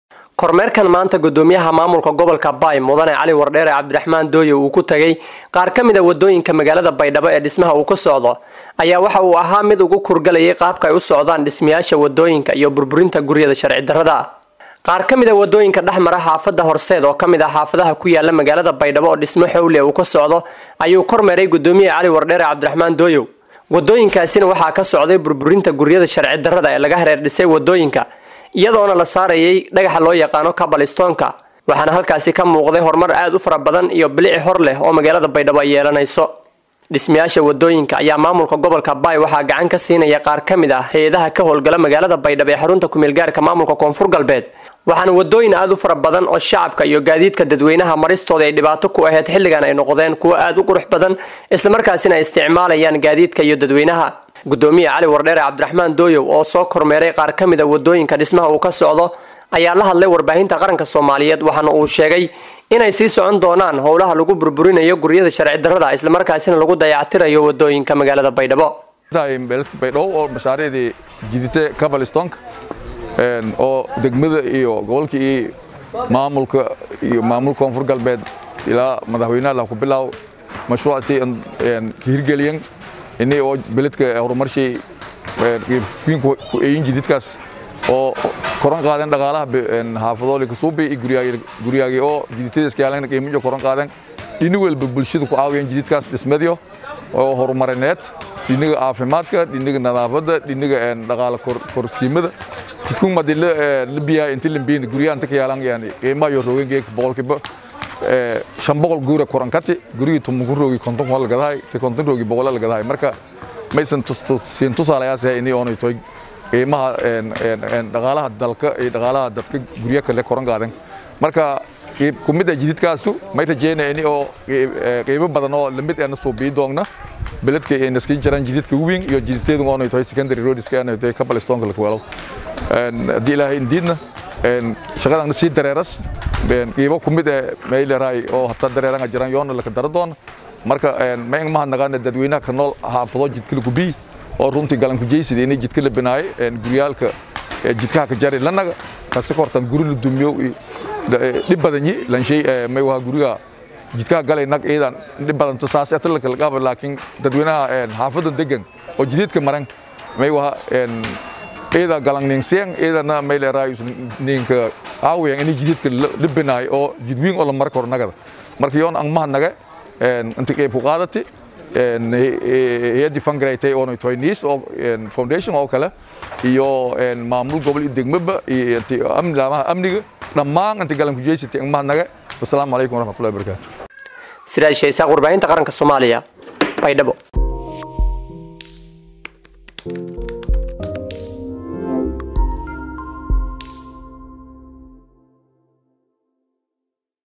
Guddoomiyaha gobolka Baay oo kormeer ku sameeyay dhismaha waddooyinka dhex mara Baydhabo.
Guddoomiyaha gobolka Baay ayaa kormeer ku sameeyay dhismaha qaar ka mid ah waddooyinka dhex mara magaalada Baydhabo.